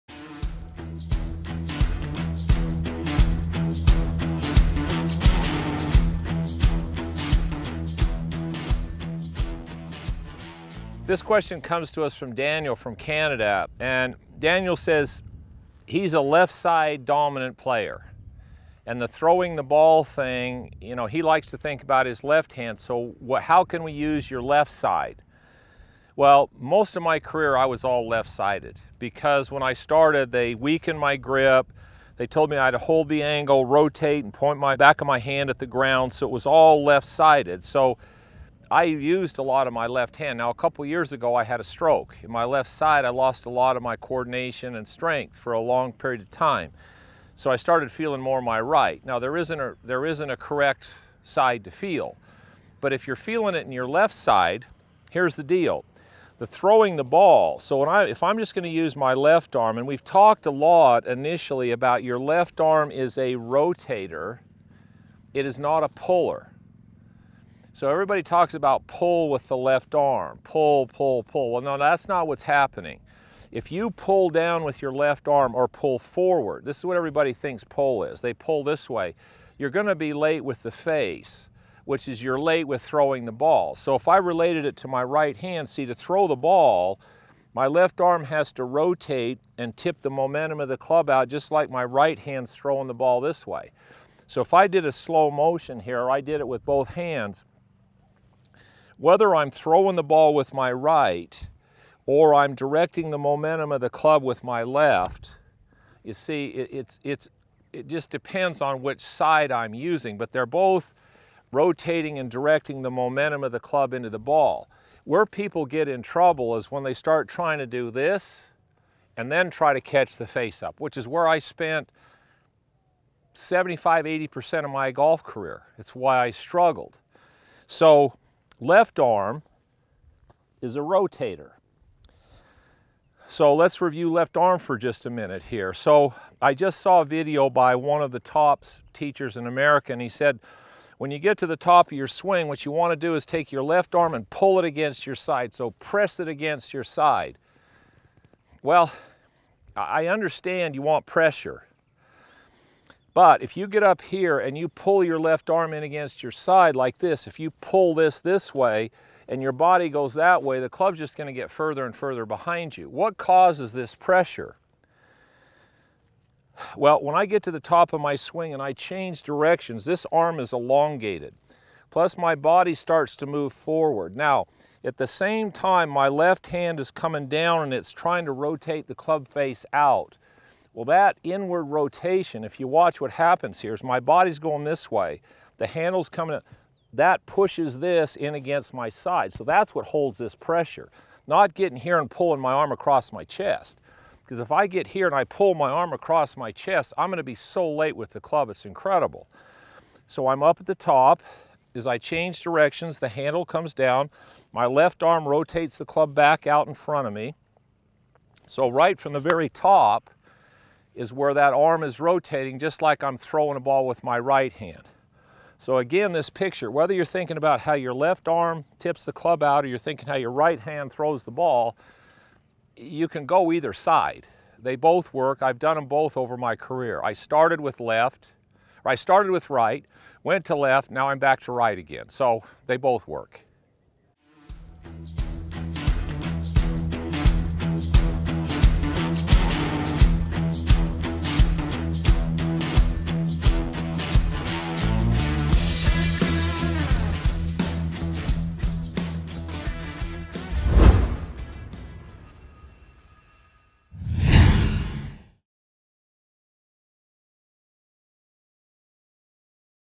Malaska Golf // Left Arm Motion - Full Swing - Left hand vs. right hand
I get a lot of questions about whether your right "throw' or left arm are the dominant in the golf swing. The answer is that it depends on where you're most comfortable. I've been on both sides in my career.

As long as you know how your hands work through the golf swing, you can get a gauge on which one has the right feel for your swing.

Don't just play golf. Understand it. Watch the complete series at malaskagolf.com.